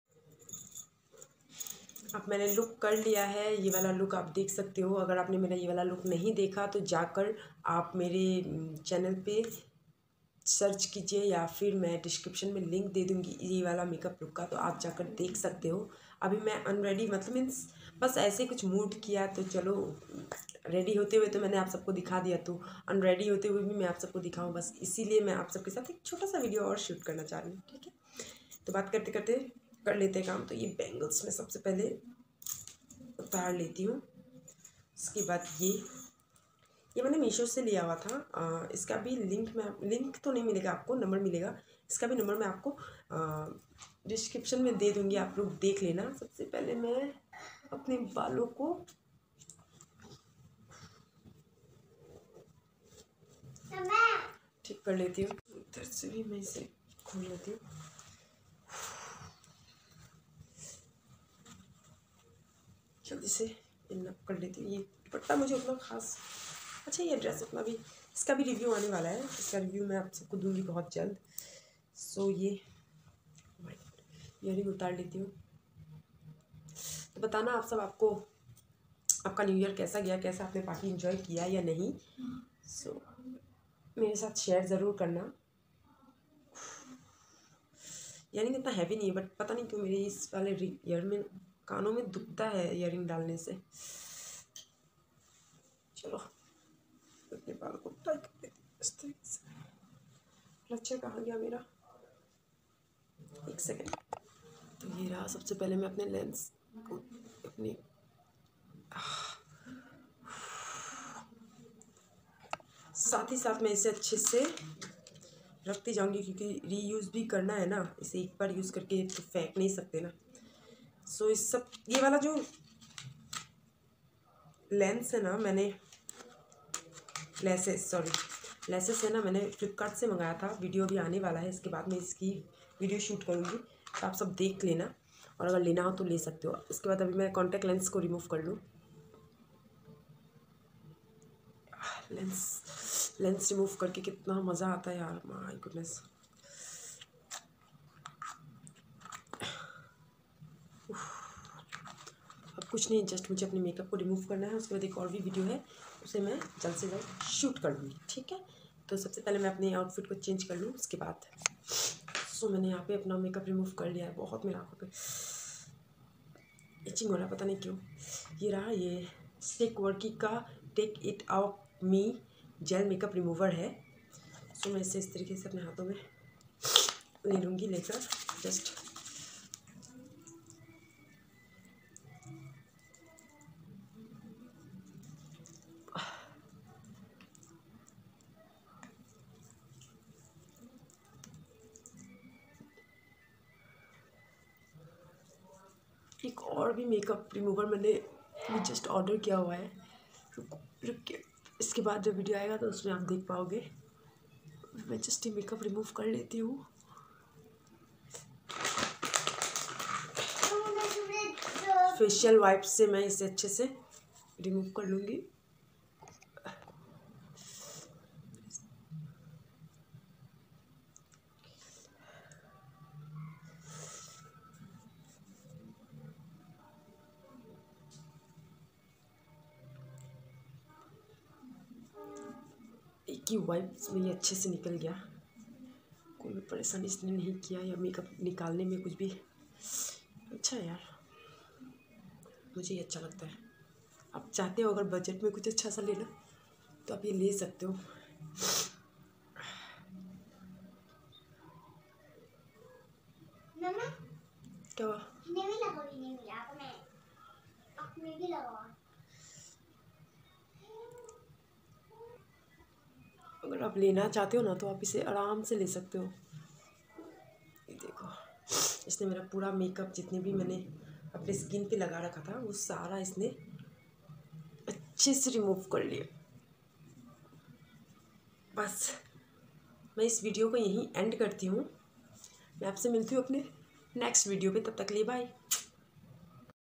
get unready with me कुछ बातें शादी करें 😉😉
hello guys I hope aap sabko meri video pasand a rahi hai agar pasand aaye rahi hai to kisi bhi channel ko subscribe main is video mein apni makeup ko kaise remove kare vahi bataya hai aap sabko